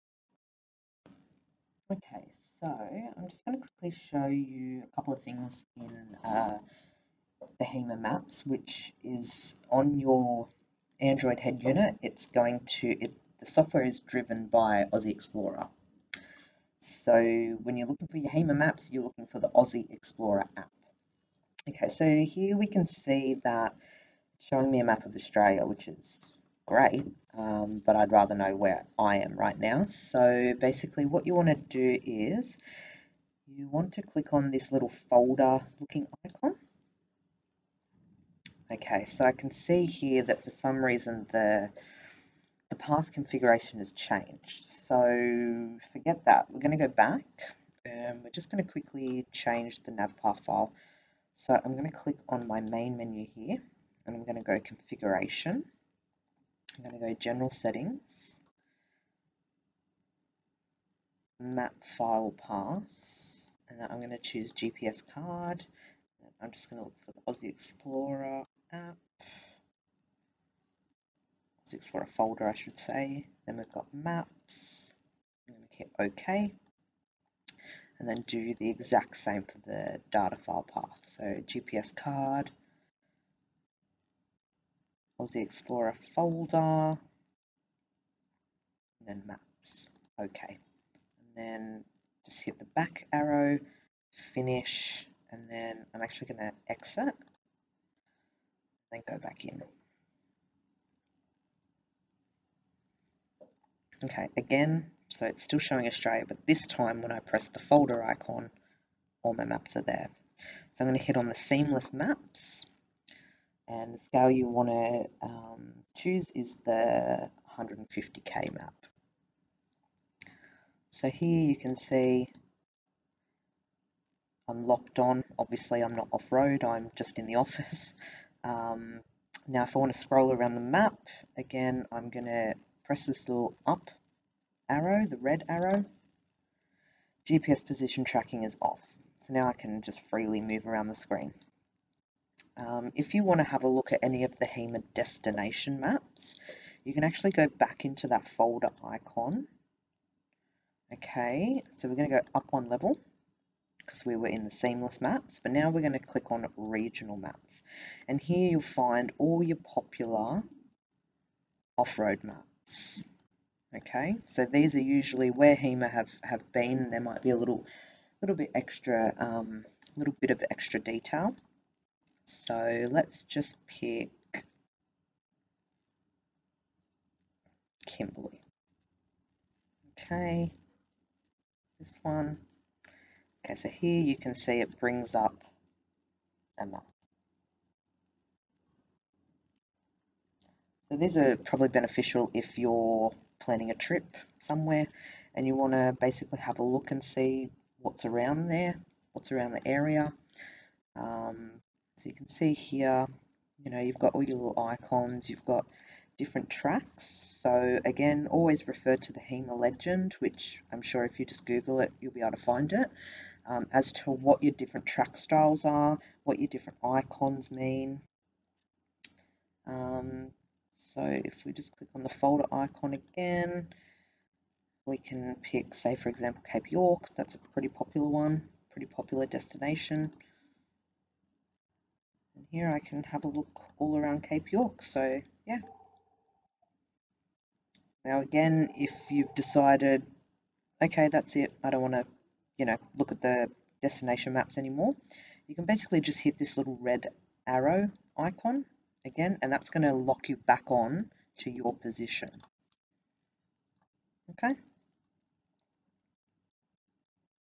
Polaris Tutorial - How to load your seamless and regional maps on Oziexplorer Android
This Tutorial will show you how to set up your nav path files, how to load your seamless 150K map and how to load the regional (otherwise known as destination) maps.